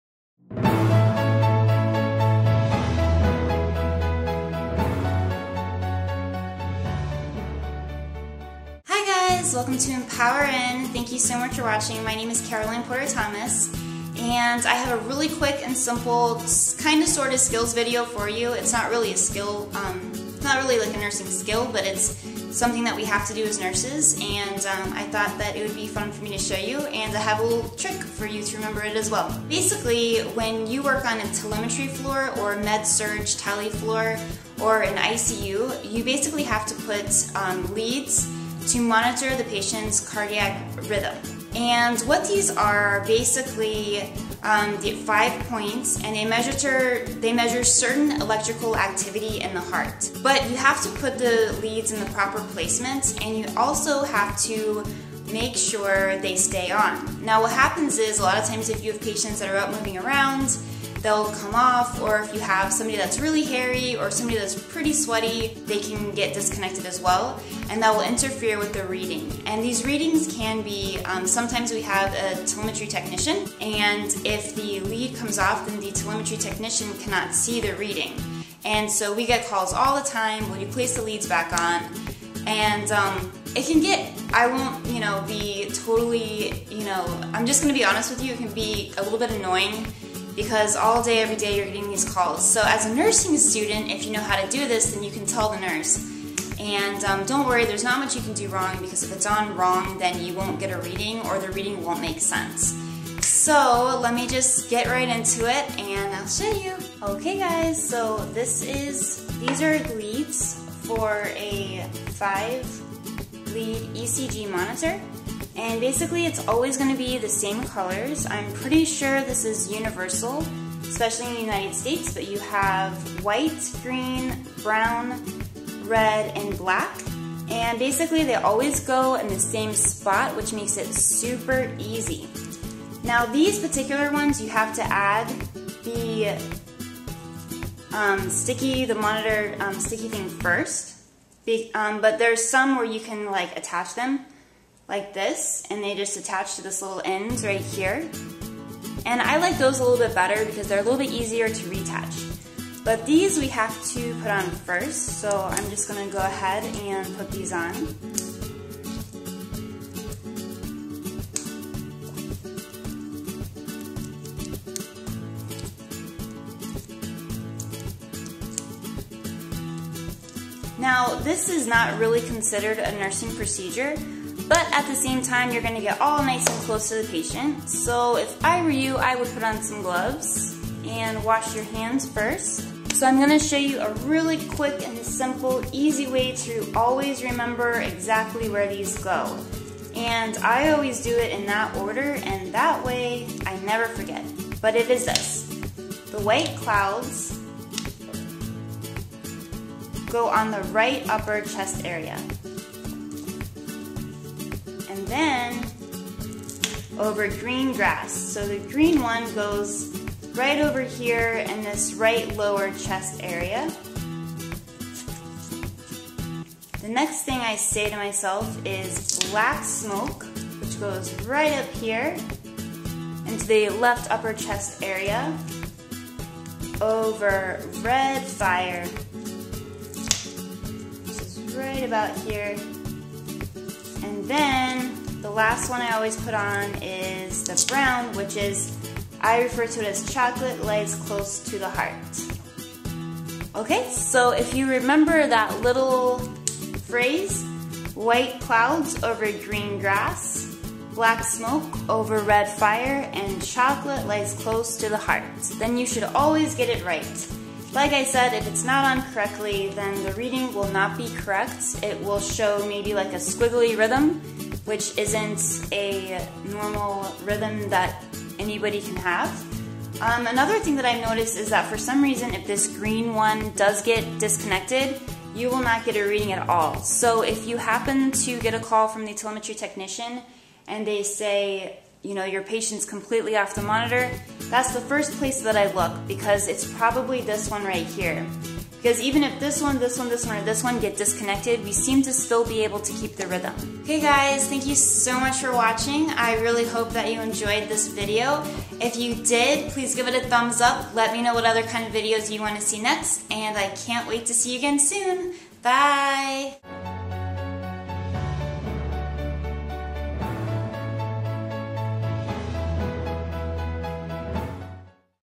Cardiac Monitor (Telemetry) Lead Placement ;)
Tele Lead Placement
Hi Guys!
This is a quick fun video to show you a little trick about how to place cardiac monitor leads also known as telemetry leads!
When you are a cardiac nurse also known as a step down nurse, tele nurse, ICU nurse. ect.
You will need to monitor your patients heart rhythm.
To do this we use what is called a ecg machine or electrocardiogram... this measures the electrical activity that the heart is producing from each of the areas.
This allows you to see what is physically going on in your patients heart. What is cool about this is that it is an extra way to monitor your patient close.
Many times these machines are build in on the walls and can restrict the patients activity due to the length of the wire.
On most of the units that I work in we have what is called a remote telemetry monitor.
It is basically the size of a hand and can be placed in the patients hospital gown pocket.
This allows the patient to room around the room & most of the time the rest of the unit and we will still be able to see out patients cardiac rhythm.
So in this video I will show you how and where to place the leads so that you can properly monitor your patient.
I will also show you a trick that will help you remember where the proper placement is :)
I really hope you enjoy this video... if you do.
Cannot wait to see you again soon!
xoxo
- Caroline